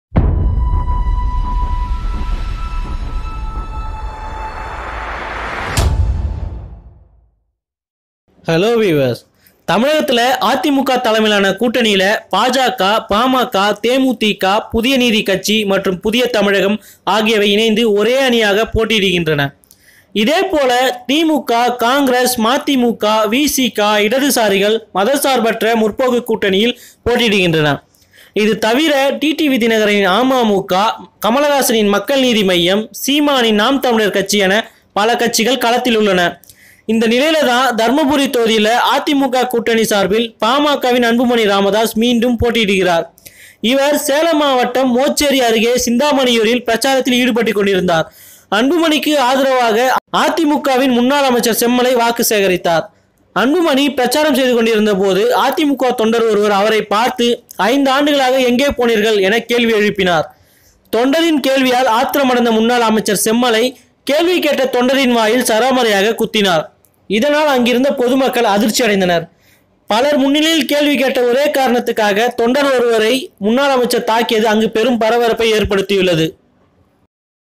அன்புமணி பிரச்சாரத்தில் கேள்வி கேட்ட அதிமுக தொண்டரை கும்மாங்குத்து குத்திய அமைச்சர்
WaitAndSee அன்புமணி பிரச்சாரத்தில் கேள்வி கேட்ட அதிமுக தொண்டரை கும்மாங்குத்து குத்திய அமைச்சர் செம்மலை